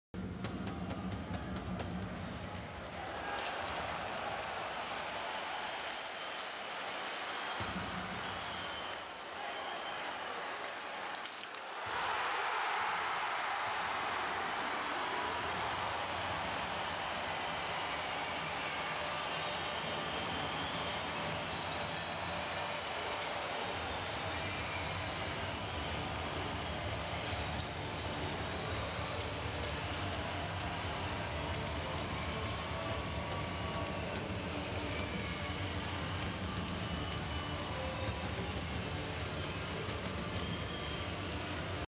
Khokhryakov rocks Radil, assists on GWG
Lovely play by Avangard's Pyotr Khokhryakov. He steals the puck off Spartak's Lukas Radil, goes to the offensive zone and then assists on Maxim Pstushko, who scores GWG